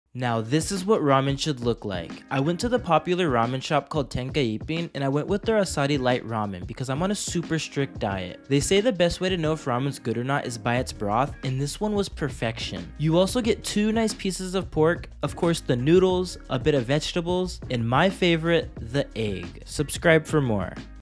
This Is What GOOD Ramen Looks Like
I went to a famous ramen shop called Tenka Ippin.
I went with their Assari light ramen because I’m on a strict diet and was not disappointed. They say the best way to know if ramen is good or not is by its broth, and this one was perfection. You also get two excellent pieces of pork, the noodles, a bit of vegetable, and my favorite… The egg.Let us know your thoughts in the comments below, and subscribe for more.

My PODCAST about Japan!